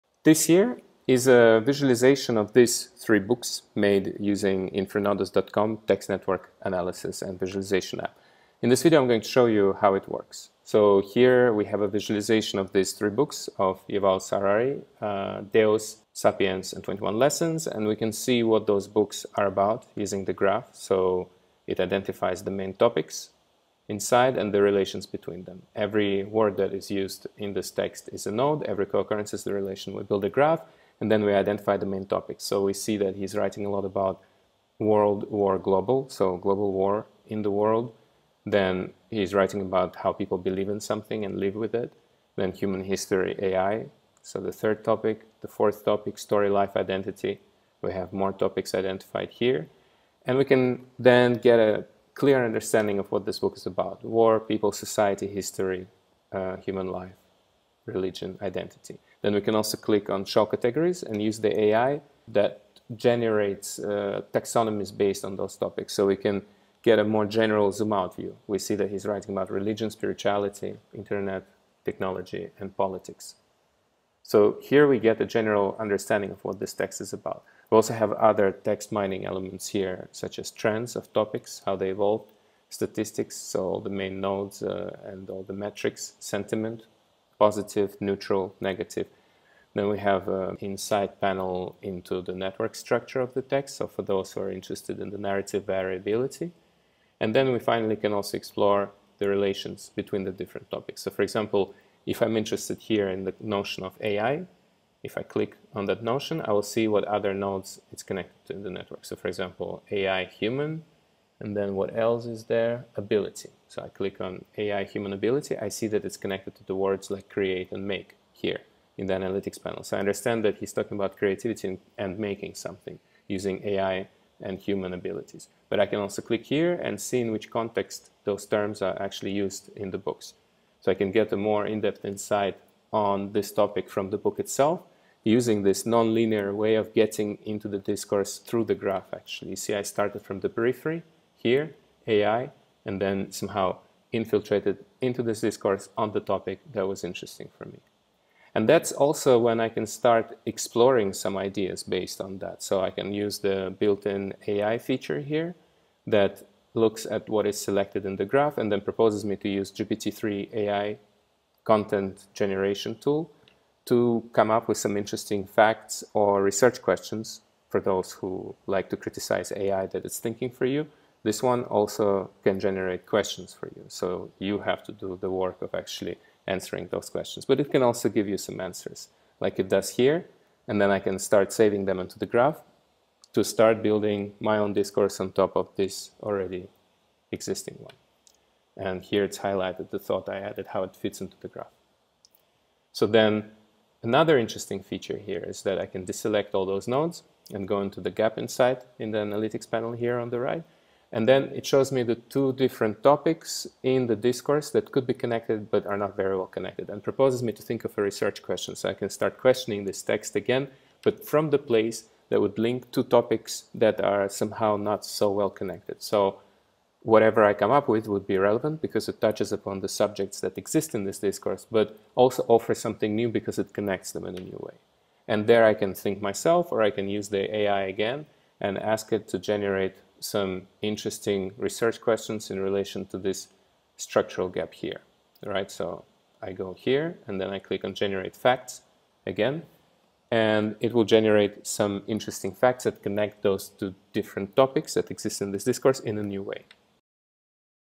Visual Text Mining using Network Science - InfraNodus.Com
This is a demo of how you can visualize and analyze any text using text network analysis tool. InfraNodus has all the powerful text mining features built-in as well as an original network graph representation that allows you to see the important patterns and relations in any text.

In this example, we use Yuval Harari's 3 books: Sapiens, Deus, and 21 Lessons. This is an outline of the workflow:

0:10 Add your content (book, text, or any other data)
0:30 Identify the main topics using the graph (topic modeling)
0:59 Categorization of main topics (taxonomy)
1:18 Topical trends, text stats, sentiment analysis
1:29 Network structure insights (variability of a discourse)
1:37 Exploring relations between different topics
2:05 Non-linear reading using the text network graph
2:28 Generating new ideas using GPT3 AI based on the graph
3:01 Saving your ideas into the graph and highlighting them
3:13 Structural gap detection = innovative ideas